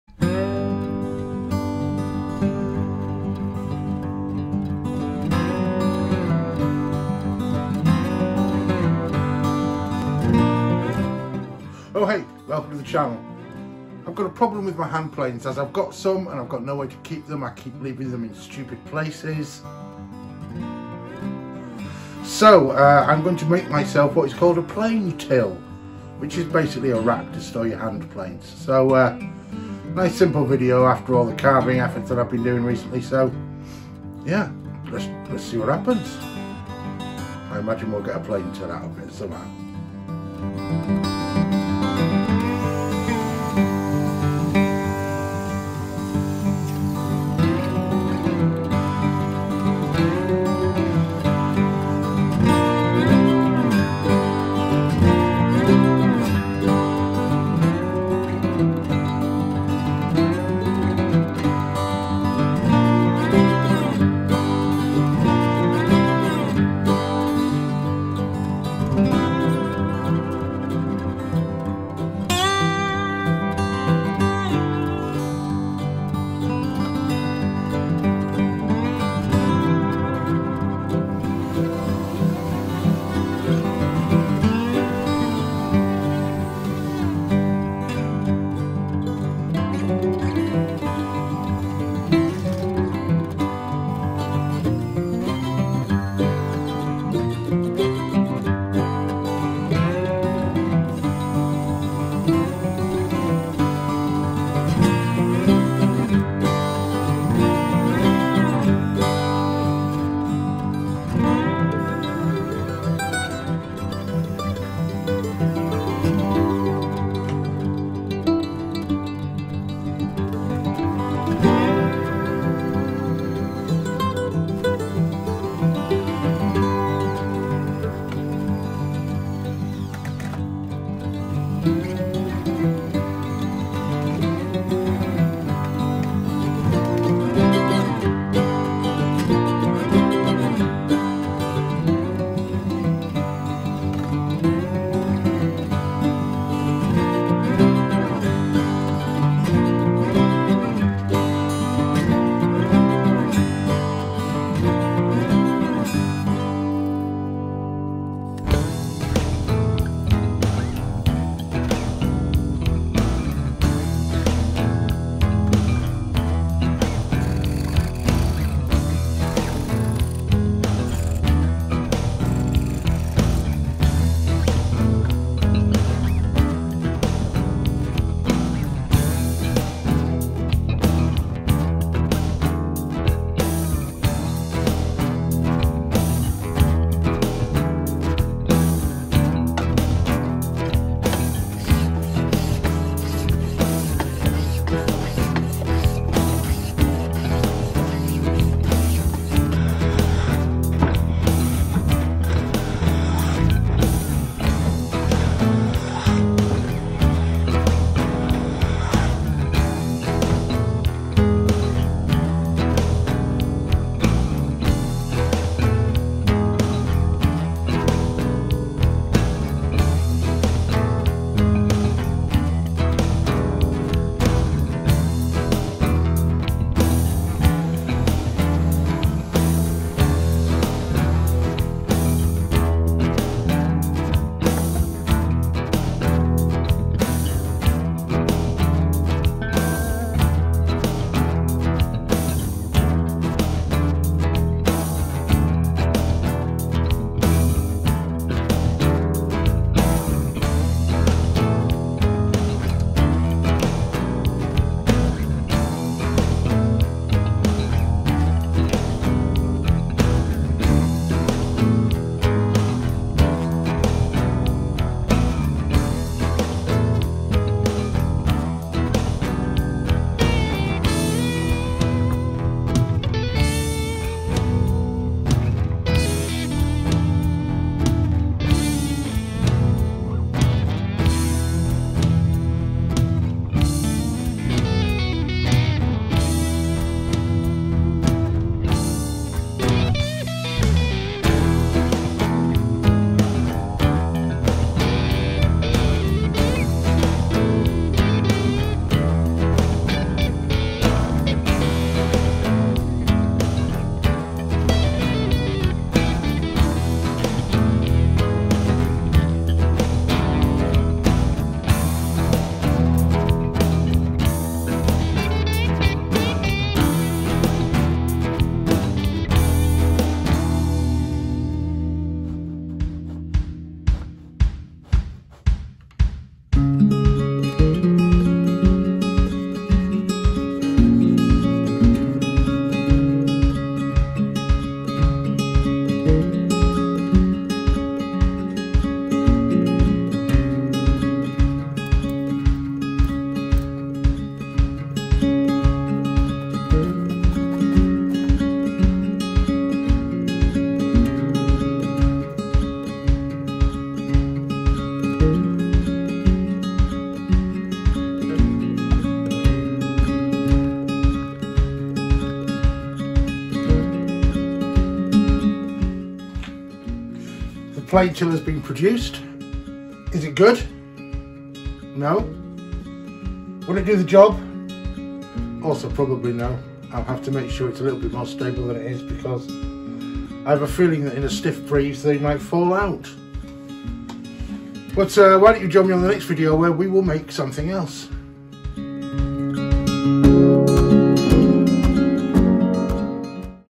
Simply Woodworking - Let's make a simple Plane Till - Pine, screws n glue
Another quick workshop storage solution, this time for my hand planes. Nowt much more to say than that!